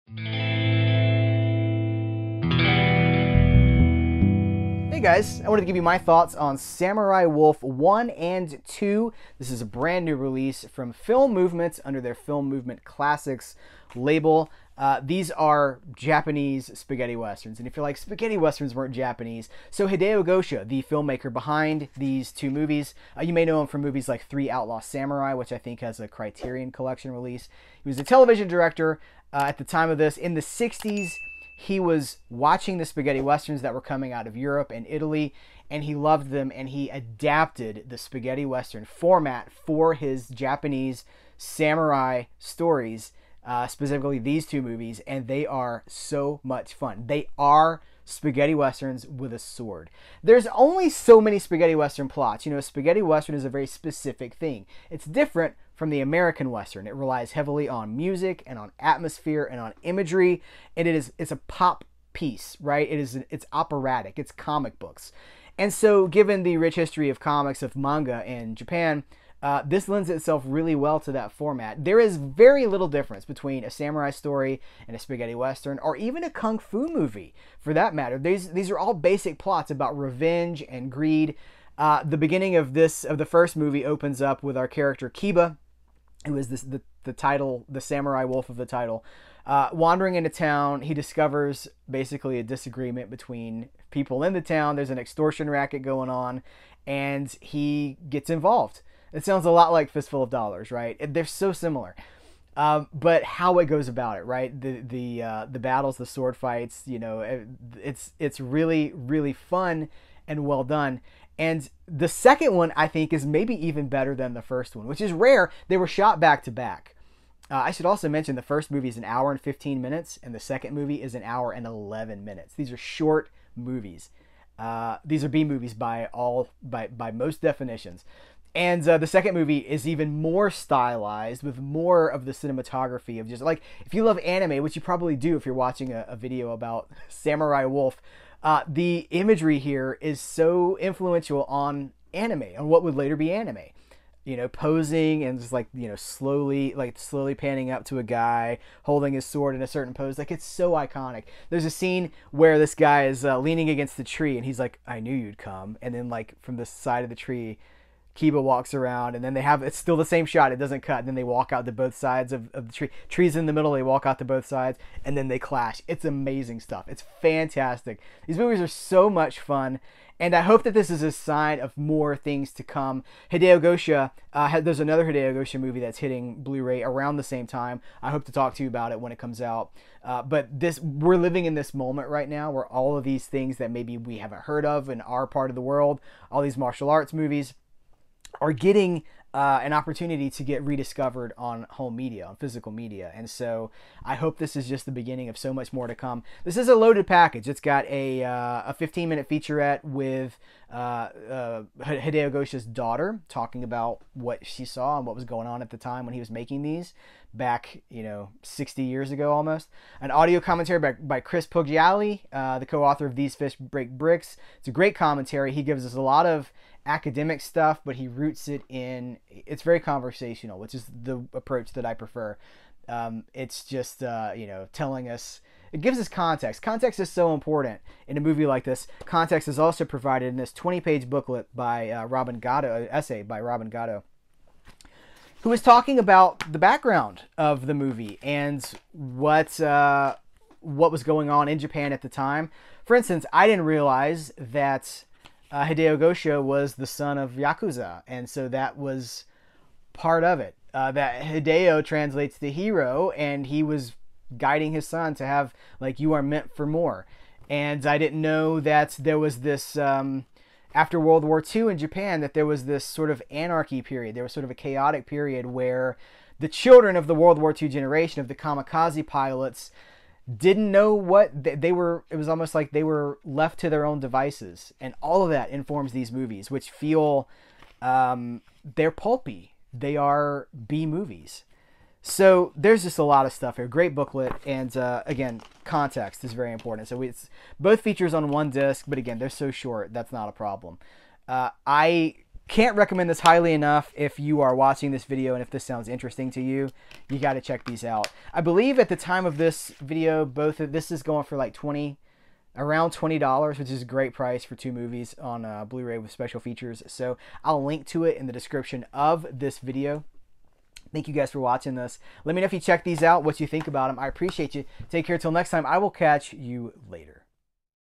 Samurai Wolf 1 & 2 | Japanese Spaghetti Westerns?
Heath reviews SAMURAI WOLF 1 & 2, which have just been released on a double-feature Blu-ray from Film Movement! With Japanese director Hideo Gosha leaning heavily into Spaghetti Western structure, these action-packed movies combine the best of East and West.

Buy Samurai Wolf 1 & 2
From Amazon

Review Archive: CerealAtMidnight.com/p/reviews.html
Ebay.com/usr/cerealatmidnight
Facebook.com/CerealMidnight
Letterboxd: CerealAtMidnite